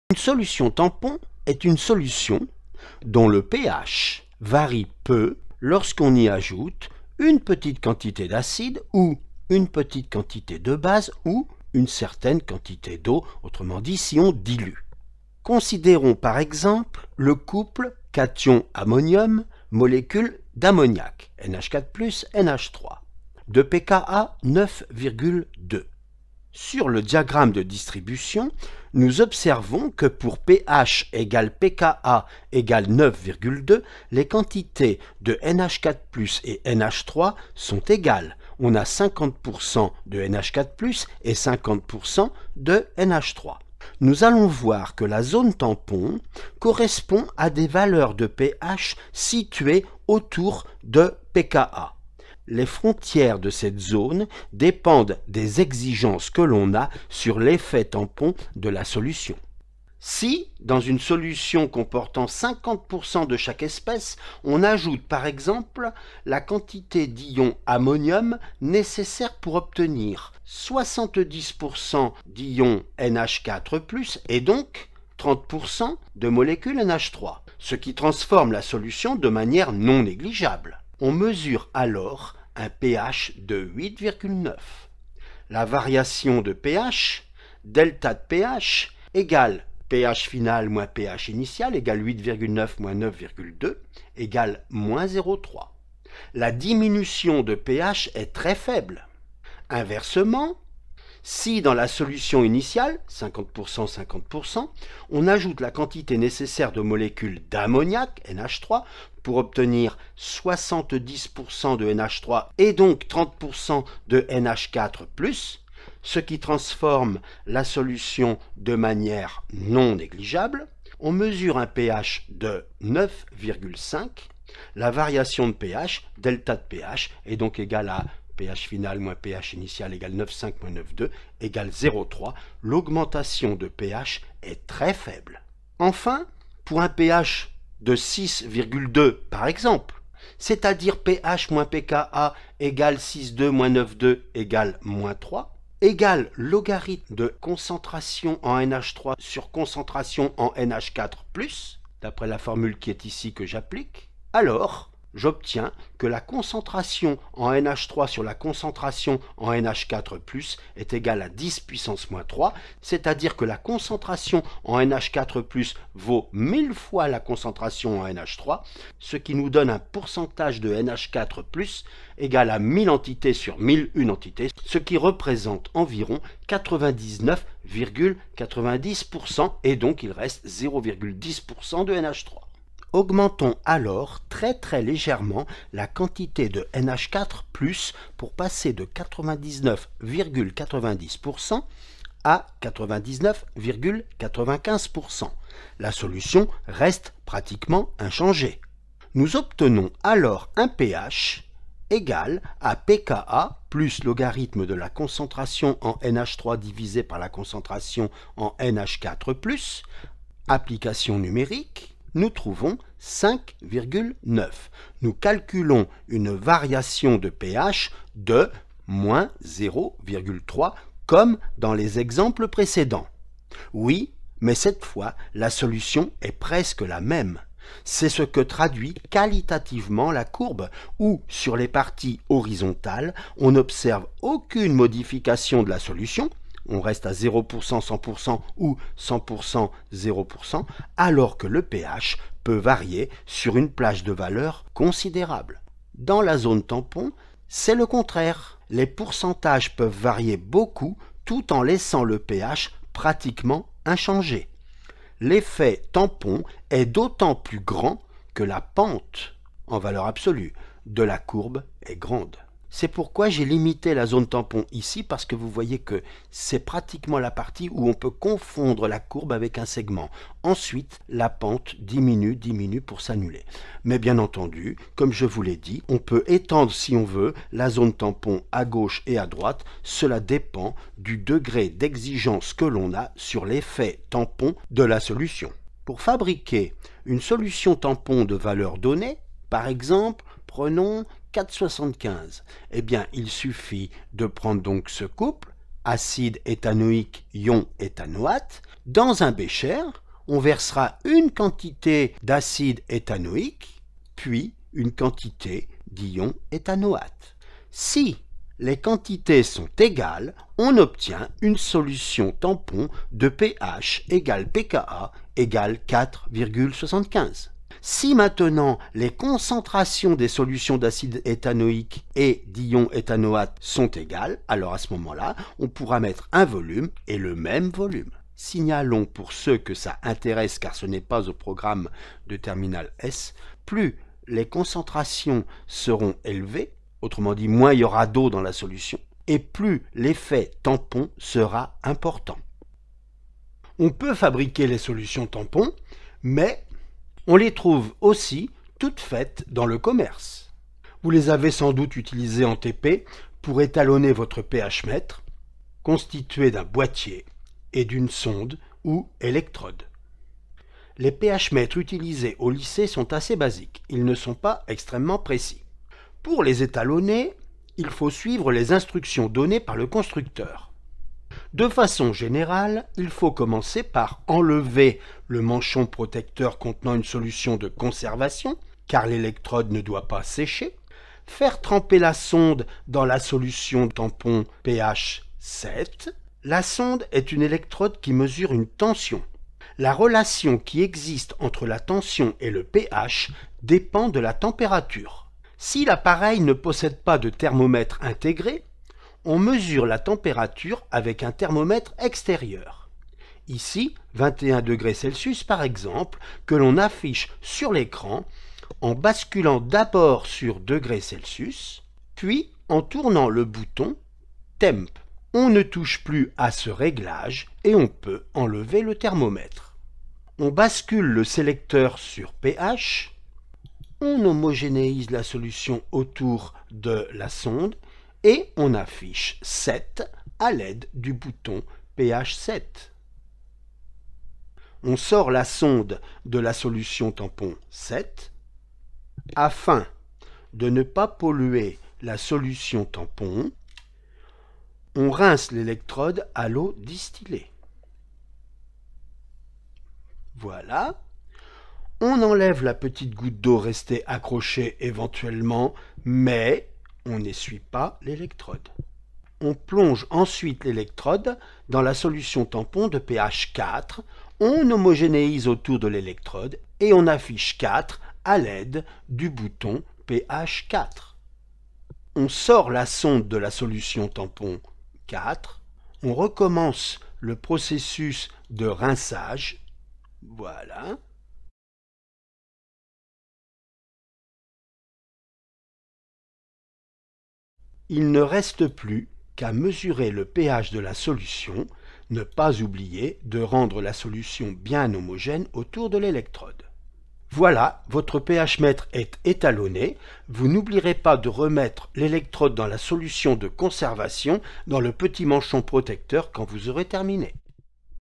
Solutions tampons
Description d'une solution tampon
Utilisation pour l'étalonnage d'un pH-mètre